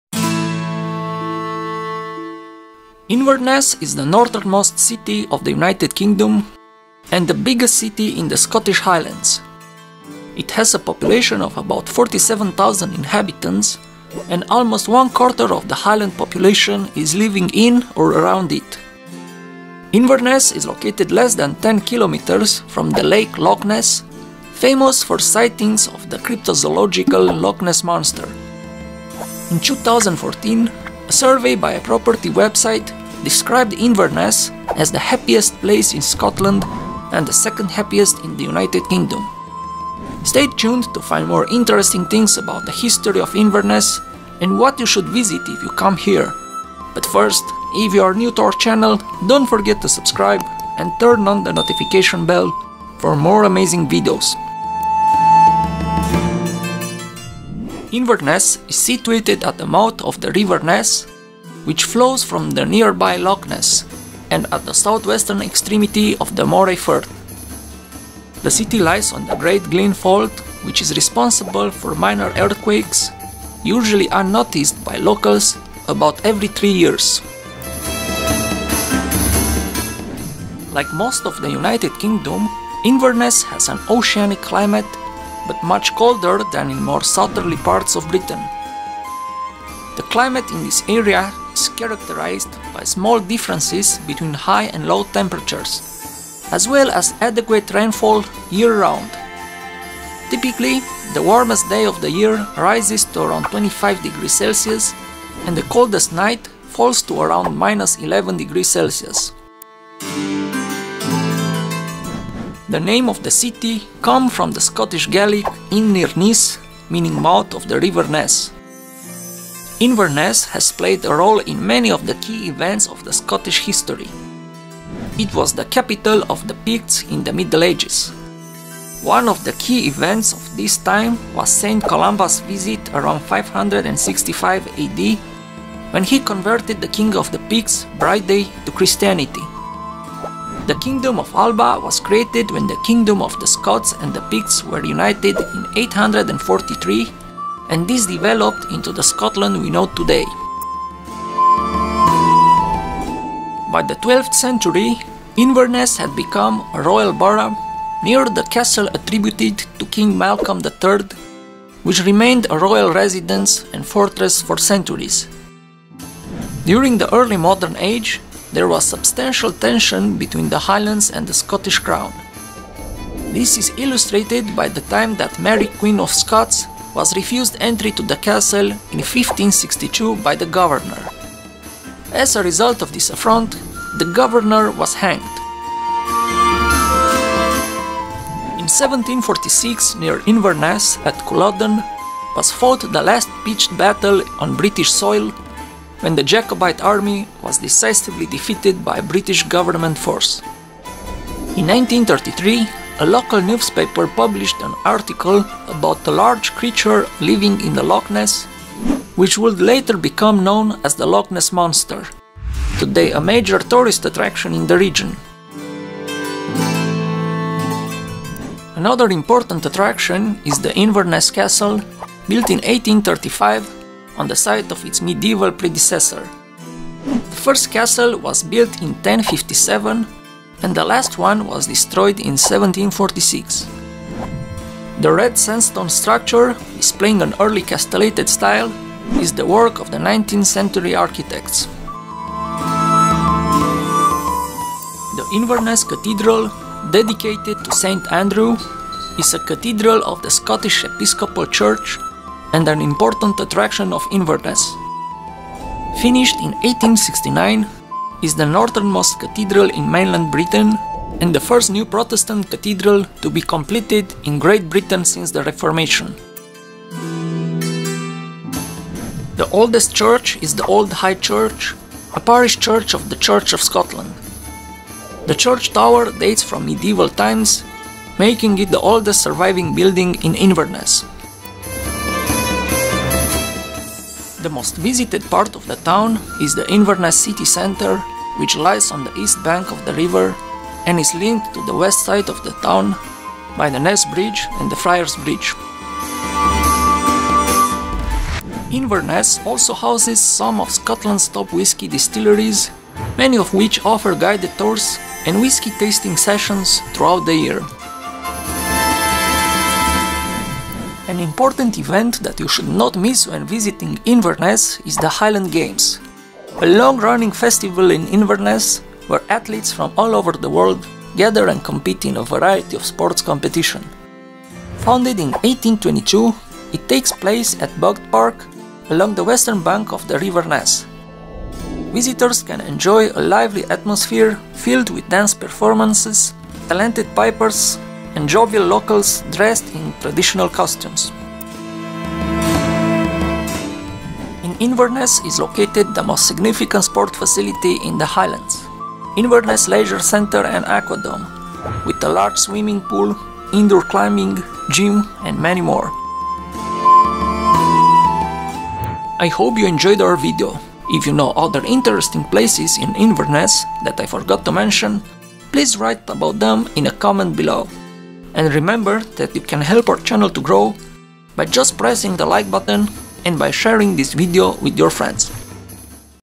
Inverness, Scotland - travel guide and history
Inverness is the capital of the Scottish administrative district of Highland. The city is located at the mouth of the Ness River in the Moray Firth. It is the northernmost "city" in the United Kingdom. The main attractions are the Inverness Castle, the Inverness St. Andrew's Cathredral, the Inverness Leisure, and the Highland Games. Inverness is only a few kilometers from the lake Loch Ness, famous for the cryptozoological Loch Ness Monster. Also, this city is considered the happiest place in Scotland.

00:00 Intro
00:57 Geography of Inverness
01:51 History of Inverness
03:21 The Loch Ness Monster
03:39 The Inverness Castle
04:05 The Inverness Cathedral
04:30 Old High Church
04:44 Inverness City Center
04:58 Inverness Whisky distilleries
05:10 Highland Games
05:44 Inverness Leisure centre and Aquadome
05:58 Outro